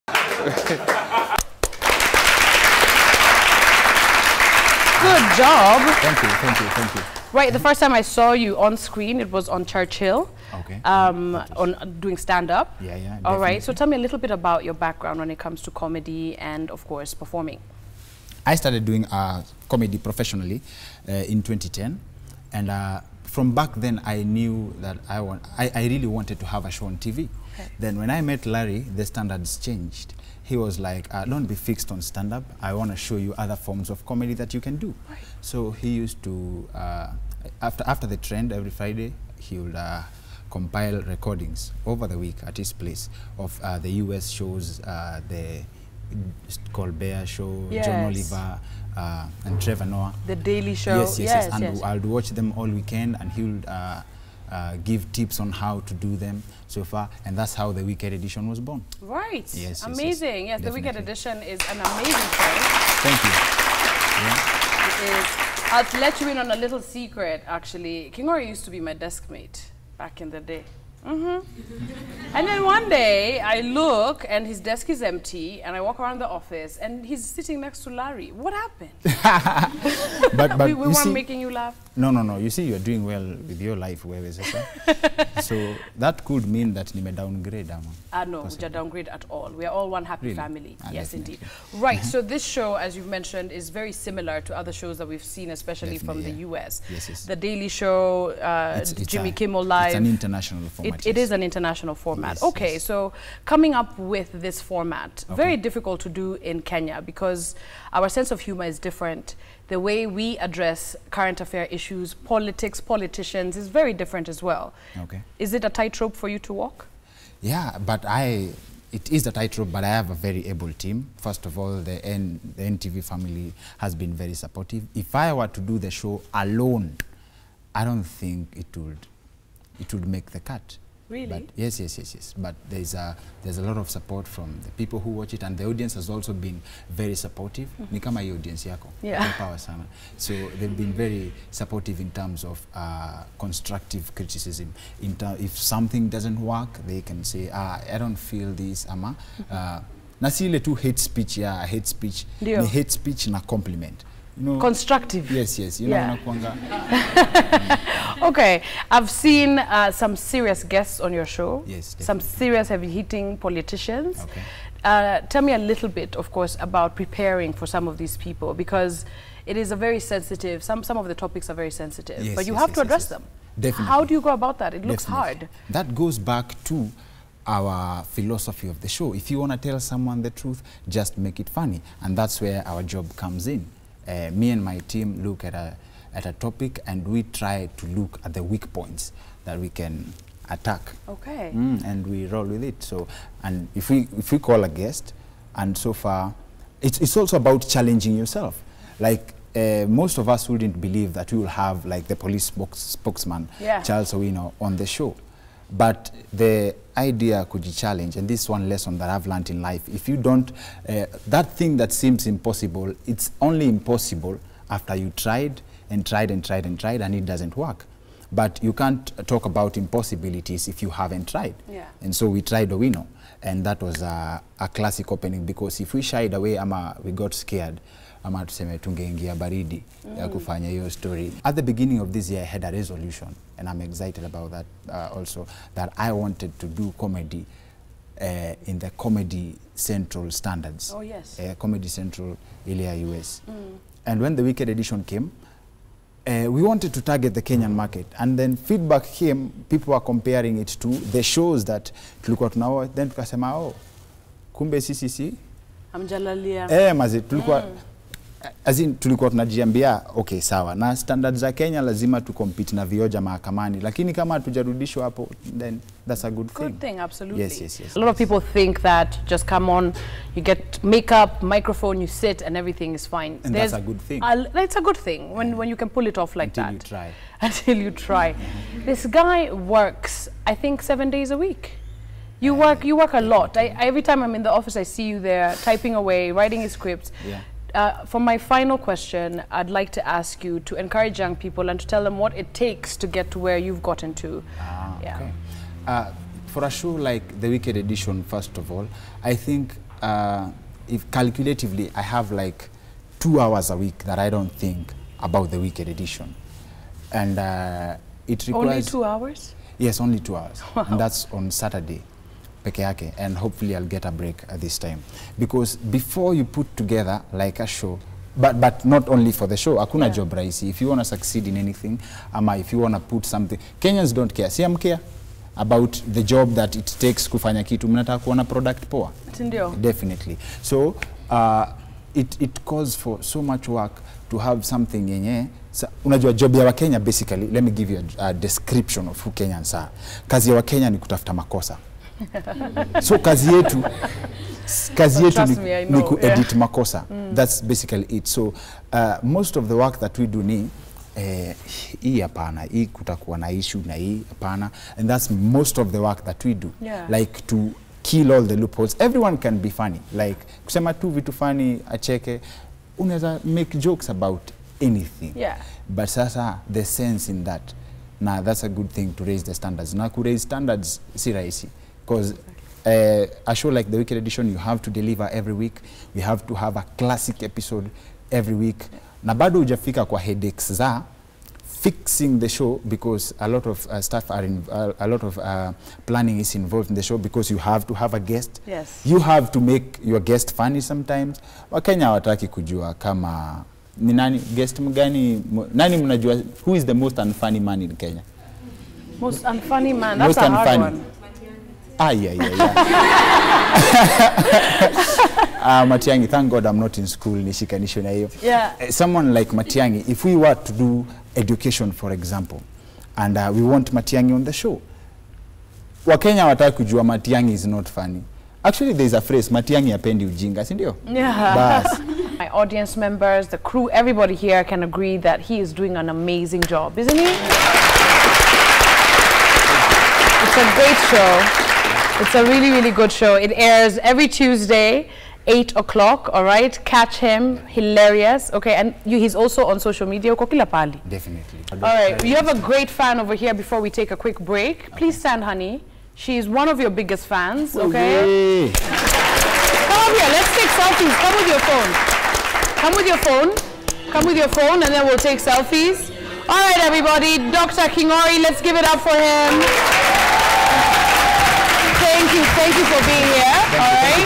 Better Living: If you want to tell someone the truth, just make it funny
Dr. King'ori on how the Wicked Edition show started.